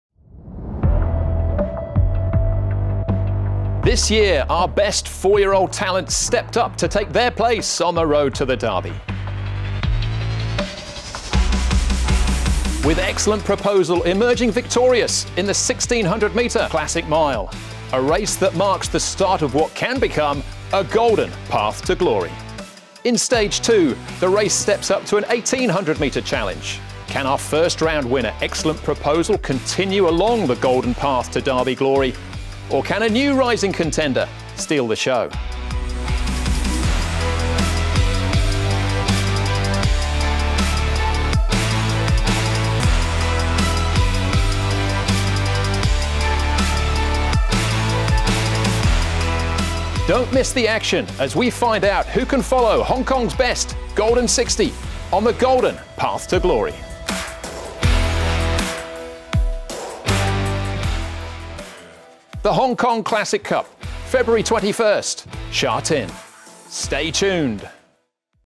[Hong Kong Classic Cup 2021] The Golden Path to Glory
The Hong Kong Classic Cup will be held at Sha Tin Racecourse on Sunday, February 21. Don't miss this second battle in the annual 4YO Classic Series, a triple-header that concludes with the BMW Hong Kong Derby in March.